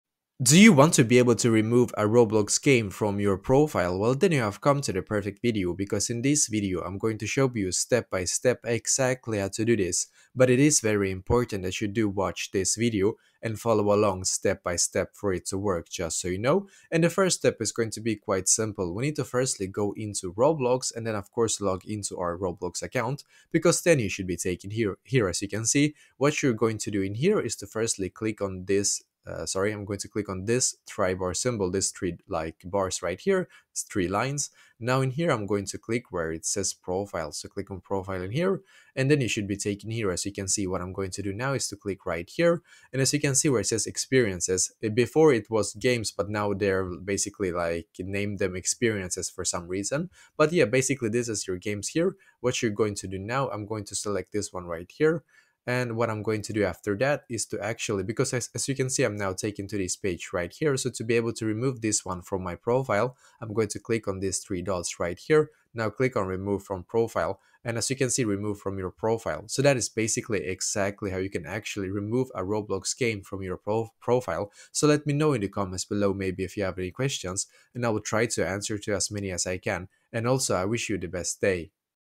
How To Remove a Roblox Game From Your Profile (2025)
If you want to remove a roblox game from your profile then this video will help you with this!